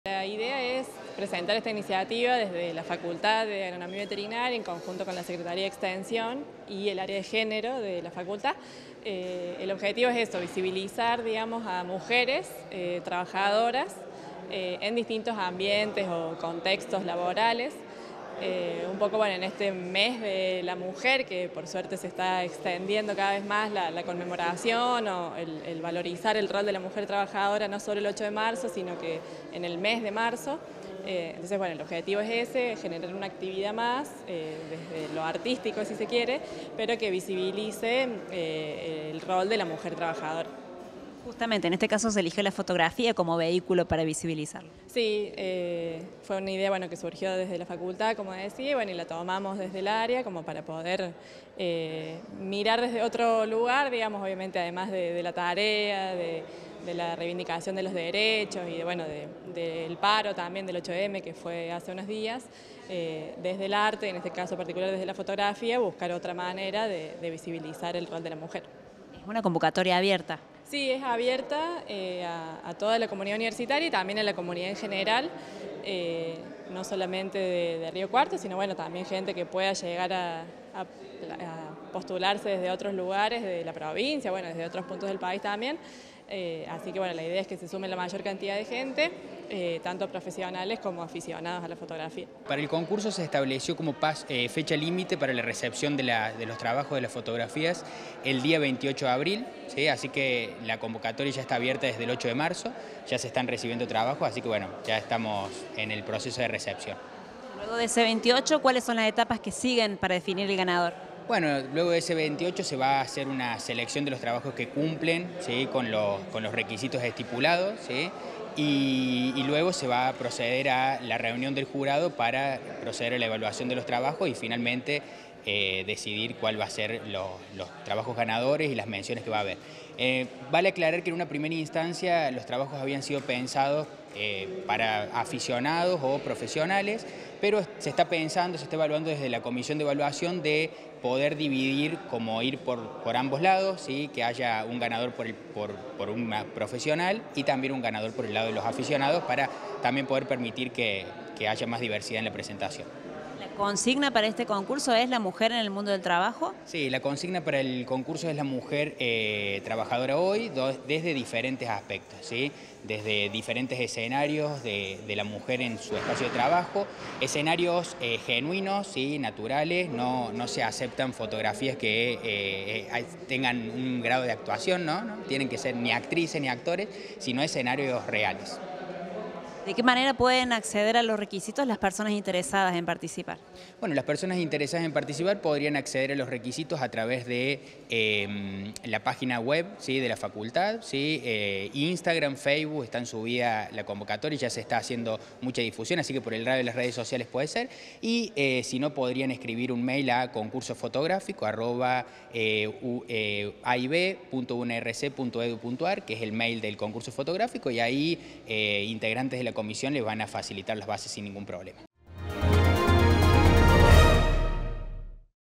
ESTÁ ABIERTA LA CONVOCATORIA PARA EL CONCURSO DE FOTOGRAFÍA “MUJERES TRABAJADORAS HOY”
CAROLINA STURNIOLO Y EMILIANO FORESTO – INTEGRANTES ÁREA DE GÉNERO FAV